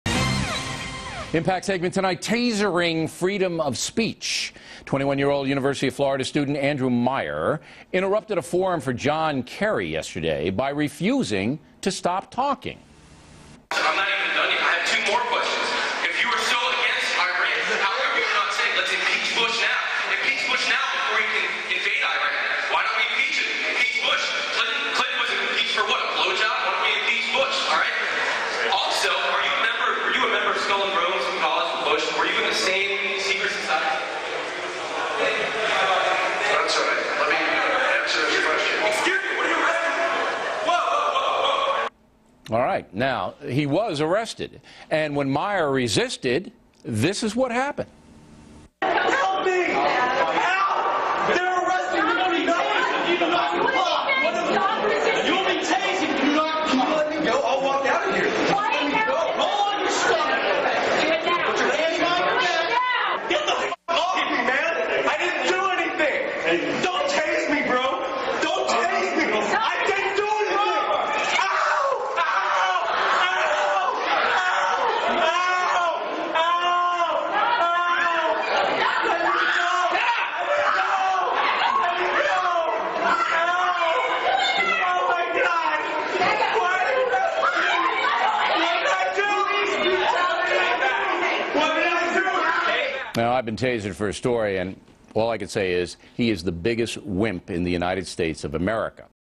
Bill O'Reilly comments on tasered student
Bill O'Reilly has a few choice words for student Andrew Meyer, who was tasered recently at a John Kerry speech at the University of Florida when he wouldn't leave as instructed to by police.